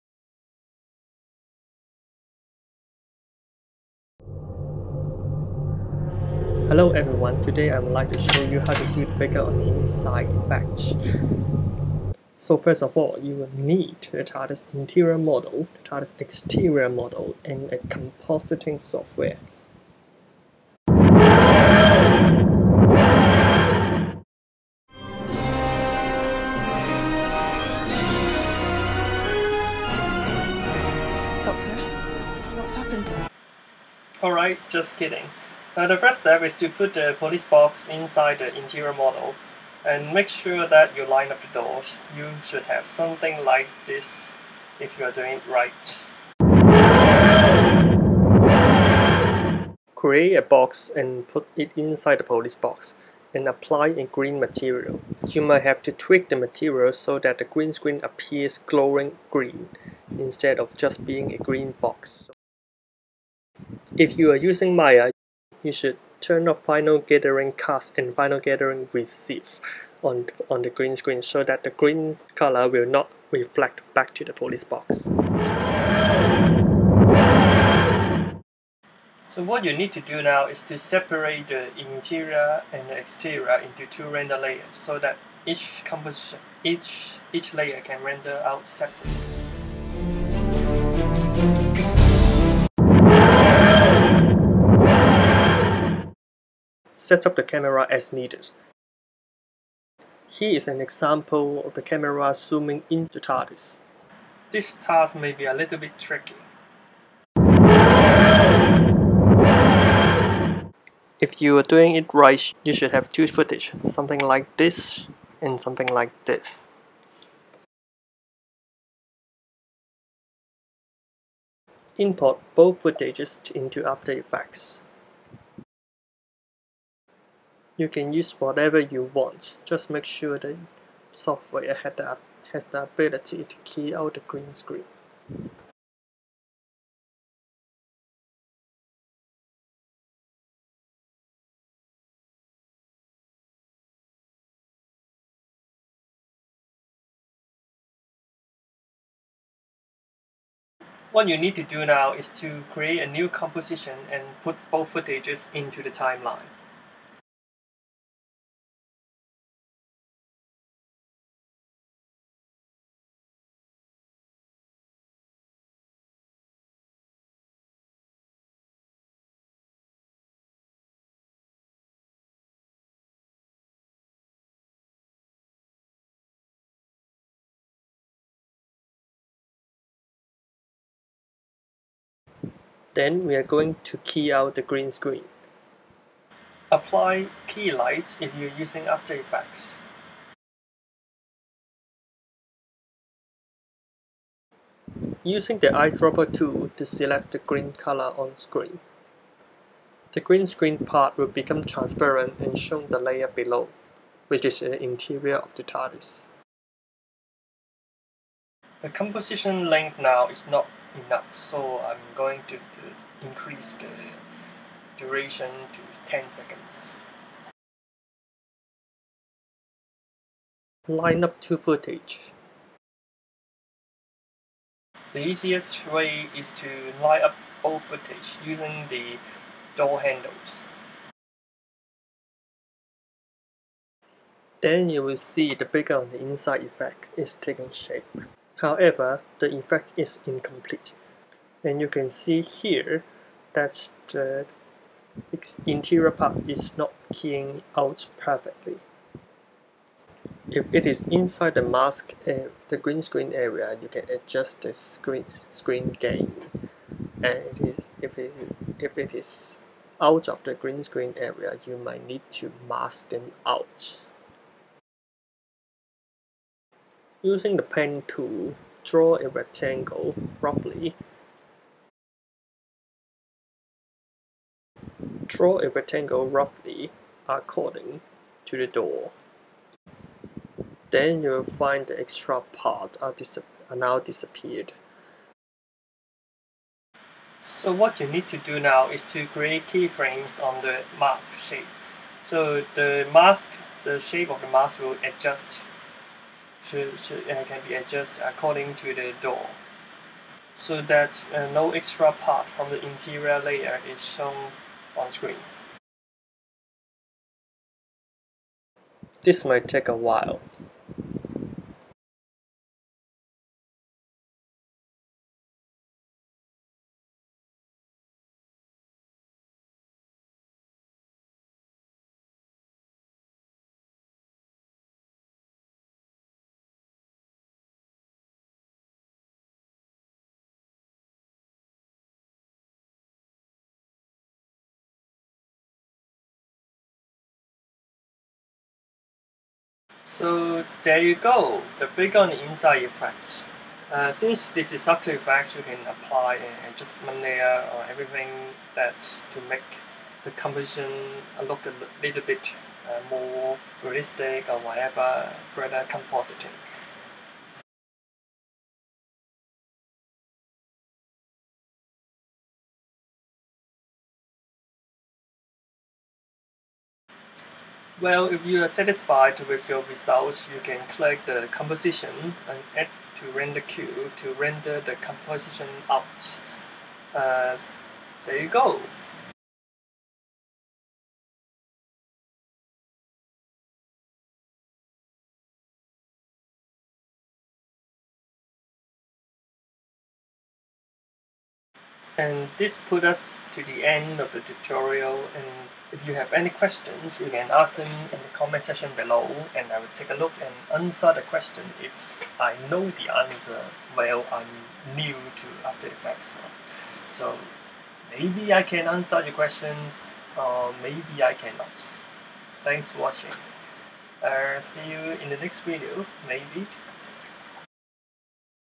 [Tutorial] Bigger on the inside effect using green screen in 3D
There are ways to do it, here's my way.

My explanation might be a little bit blurry (or shitty), if you have any question, please ask it in the comment section, I will see what I can do =]